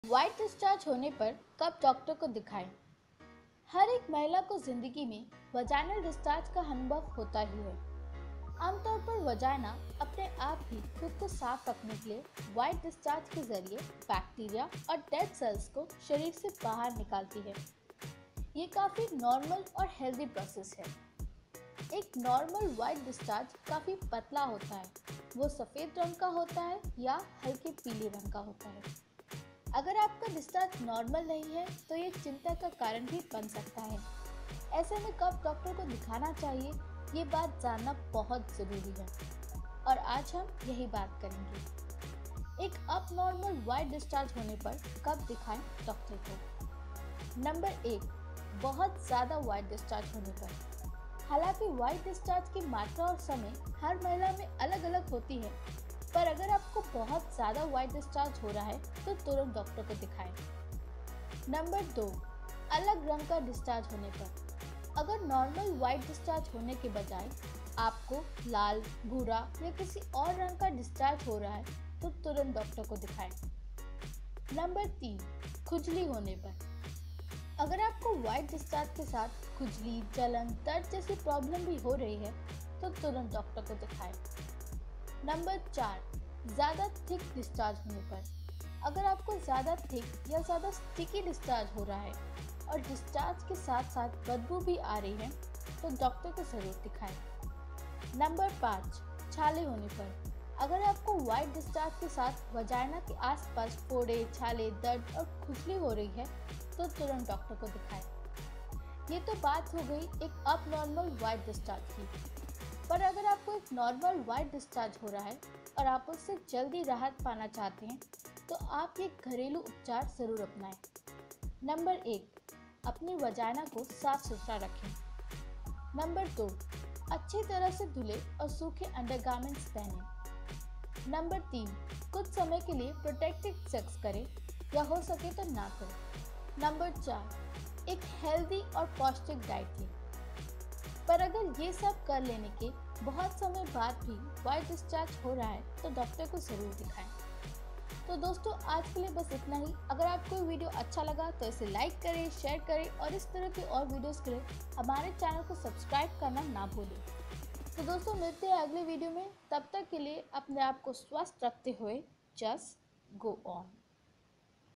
वाइट डिस्चार्ज से कैसे राहत पाएं | cure for white discharge !
हर एक महिला को जिंदिगी में वजाइनल डिस्चार्ज का अनुभव होता ही है। आमतौर पर वजाइना अपने आप ही खुद को साफ़ रखने के लिए वाइट डिस्चार्ज के जरिये बैक्टीरिया और डेड सेल्स को शरीर से बाहर निकालती है। ये काफी नार्मल और हेल्थी प्रोसेस है। एक नार्मल डिस्चार्ज काफी पतला होता है, वह सफ़ेद रंग, या हलके पीले रंग का होता है। अगर आपका डिस्चार्ज नार्मल नहीं है तो ये चिंता का कारण बन सकता है, ऐसे में कब डॉक्टर को देखना चाहिए ये बात जानना ज़रूरी है और आज हम यही बात करेंगे।